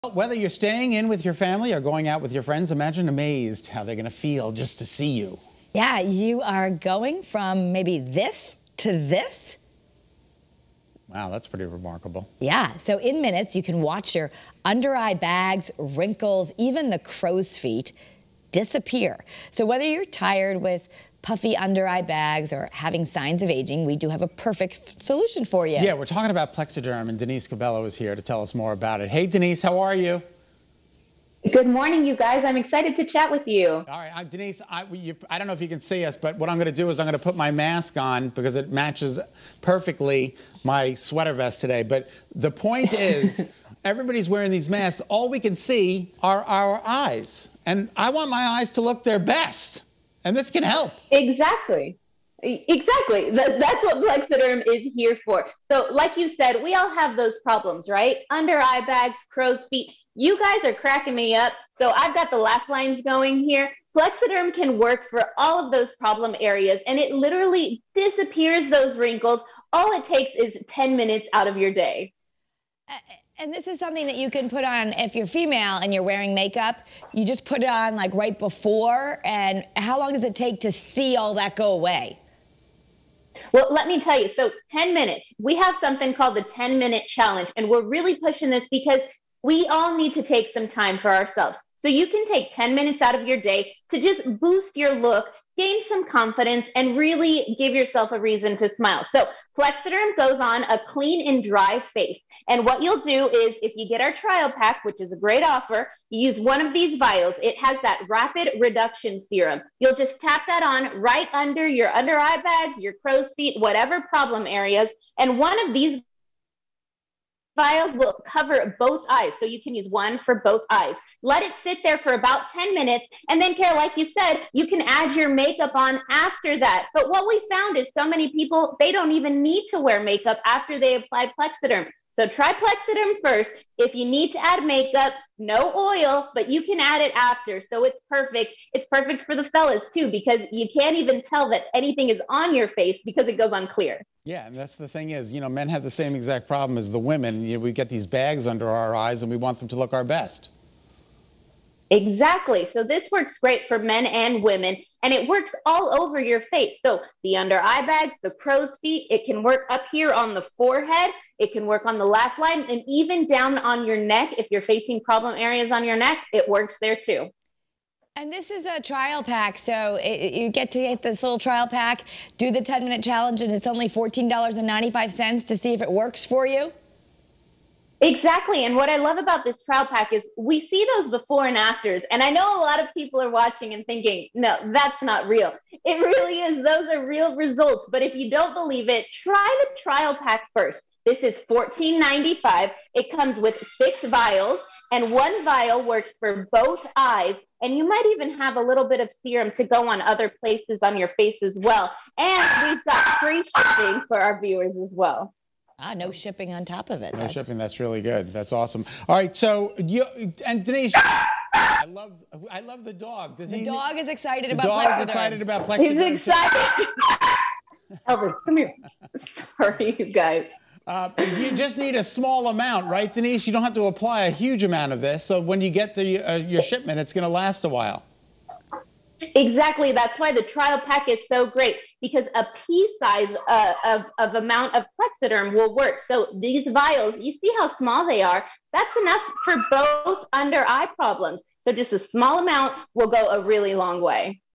Get Rid of Wrinkles
You can watch your under eye bags or wrinkles on your face vanish in minutes.  Denise Cabello is telling us about the wonders of Plexaderm!